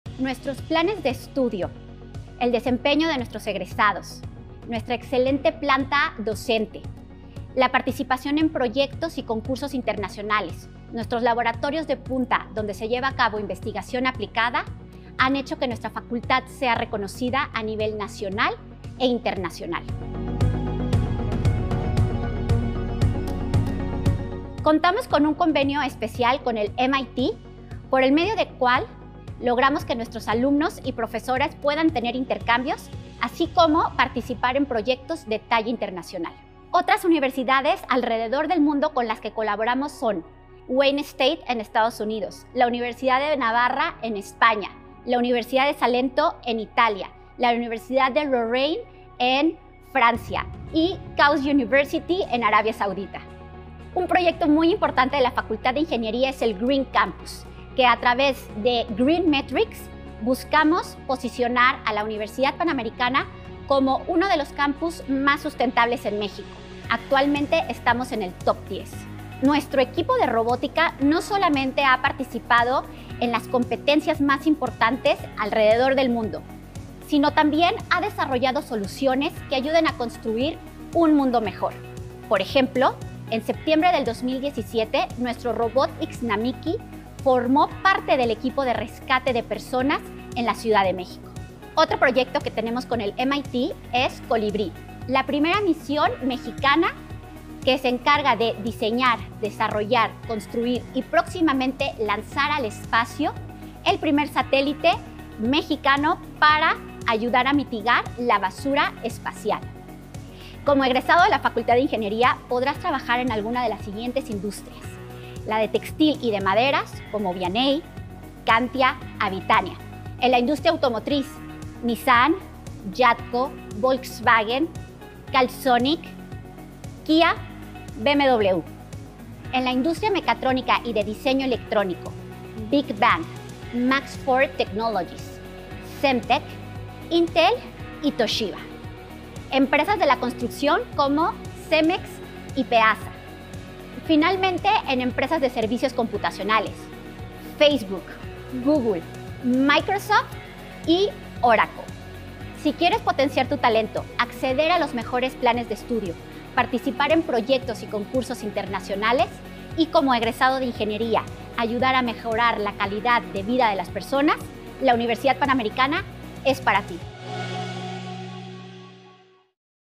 Conoce la Facultad de Ingeniería de la UP Campus Aguascalientes
Mercedes Urzúa, Directora de la Facultad de Ingeniería de la UP Campus Aguascalientes, te cuenta sobre los convenios internacionales, proyectos y reconocimientos de la Facultad.